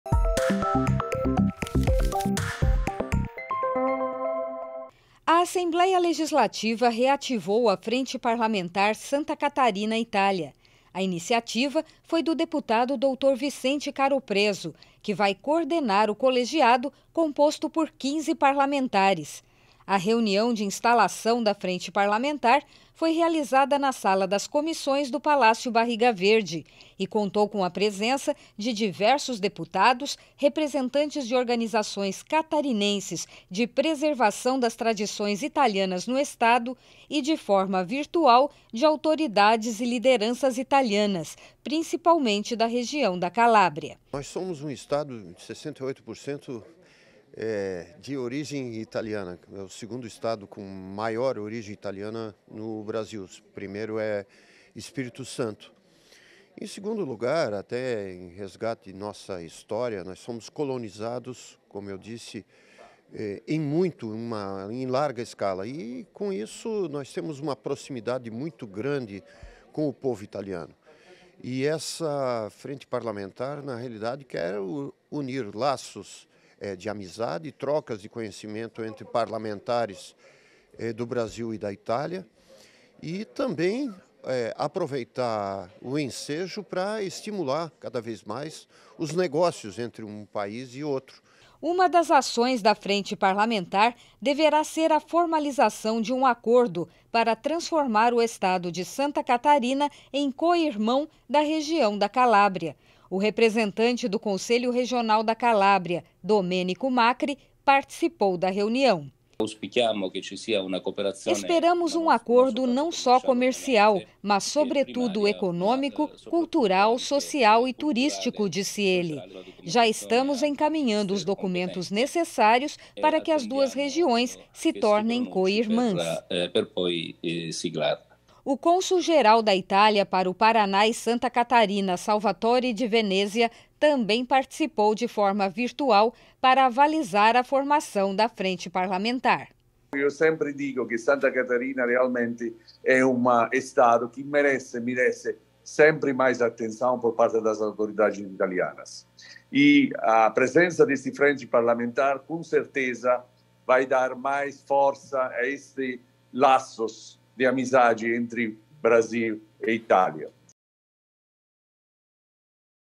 É Notícia - Alesc lança Frente Parlamentar Santa Catarina-Itália - 01/03/23
O deputado estadual Dr. Vicente Caropreso (PSDB) reativou a Frente Parlamentar Santa Catarina-Itália. O ato foi no Palácio Barriga Verde e contou com a presença, de forma virtual, de autoridades da Calábria, região do Sul da Itália. O objetivo do colegiado , que reúne 15 parlamentares, é fomentar relações econômicas, sociais, culturais e turísticas.